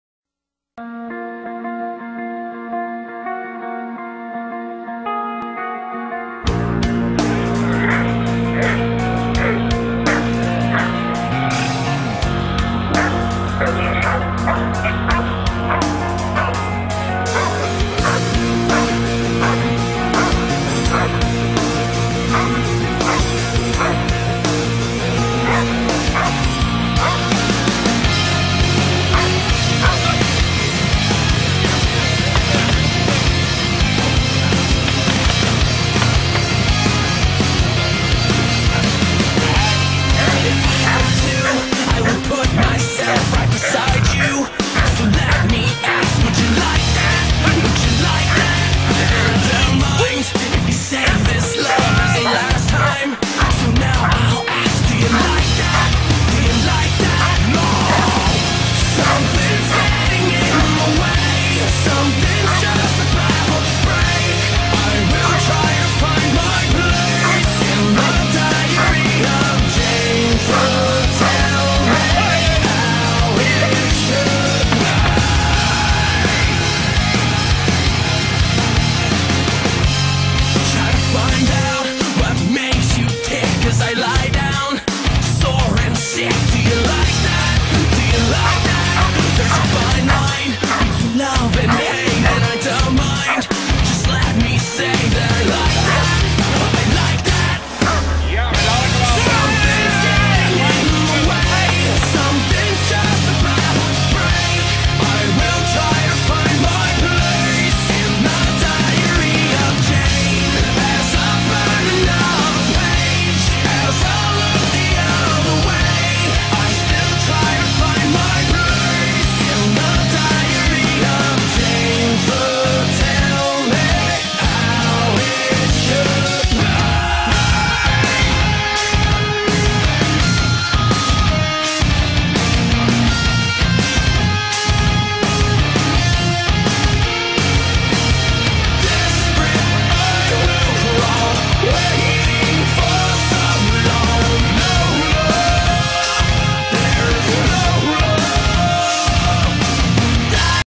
Ginger Kiridesja training 18 Juli 2011
Ginger is een zwart bruine Duitse Herder teef en in eigendom van Conny